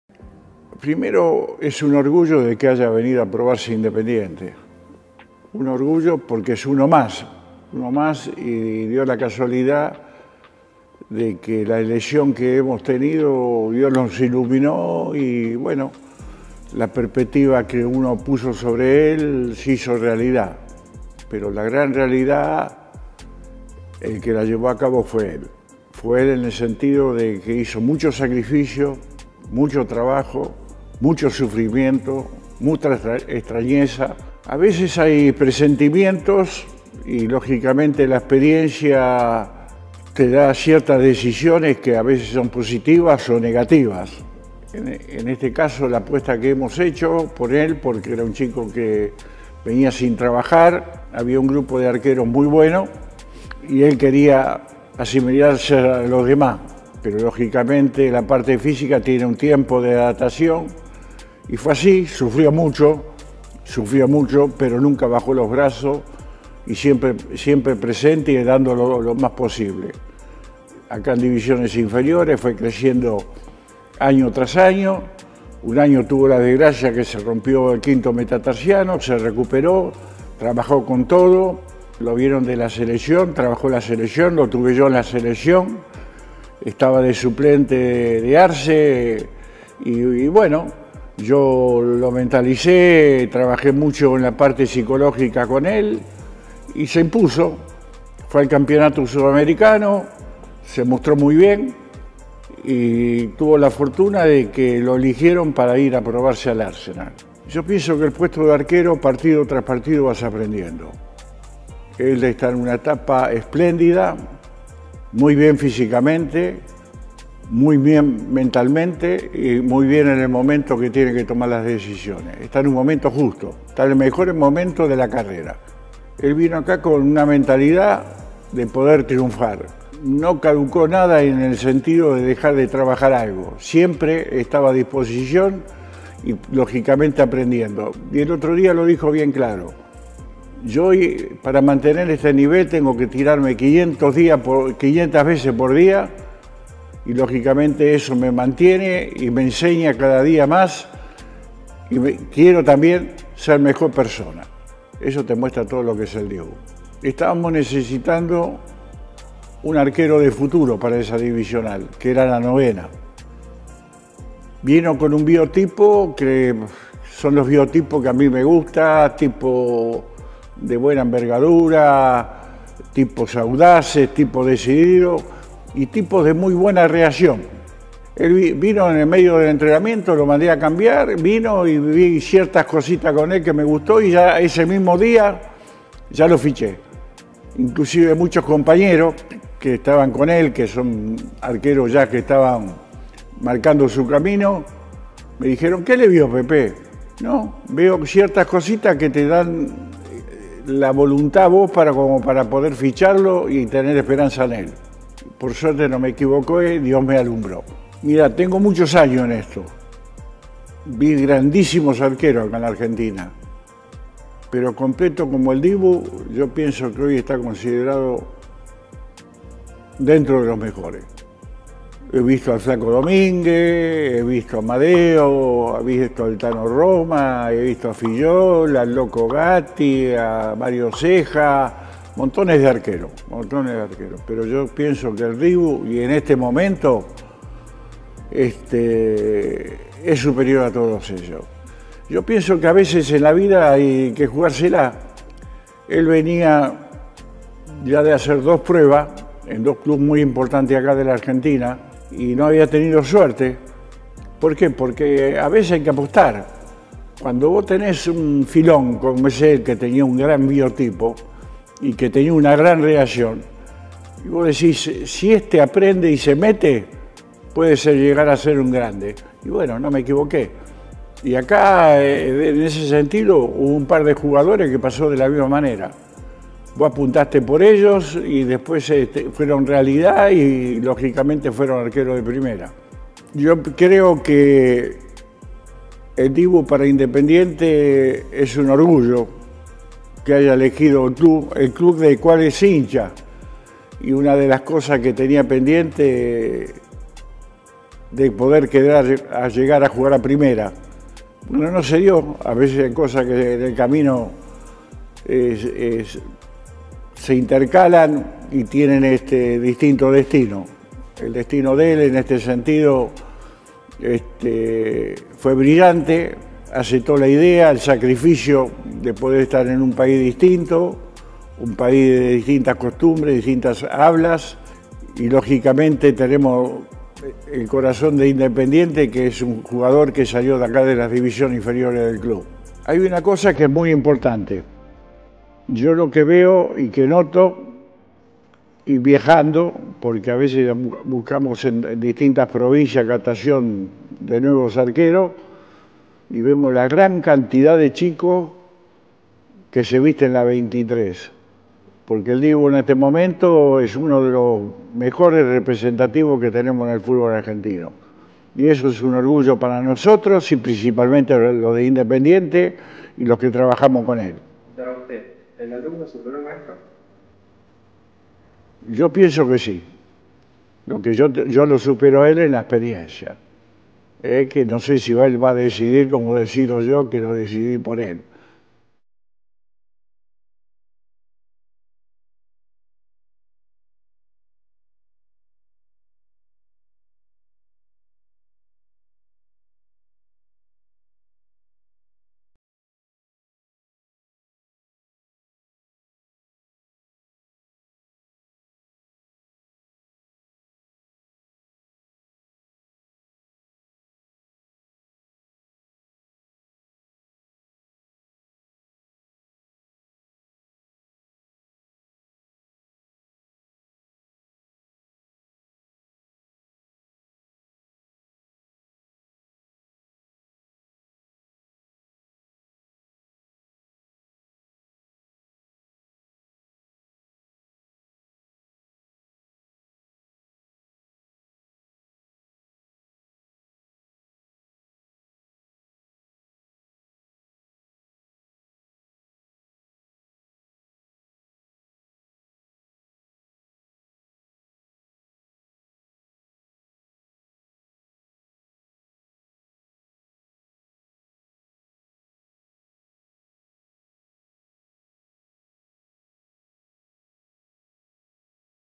Santoro: "Vi grandísimos arqueros en Argentina pero el Dibu es superior a todos ellos"
"Tengo muchos años en esto y vi grandísimos arqueros en Argentina. El Dibu, en este momento, es superior a todos ellos", dijo el gran formador e ídolo de Independiente sobre el arquero de la Selección. Escuchalo hablar del momento en que lo descubrió, sus inicios y su formación.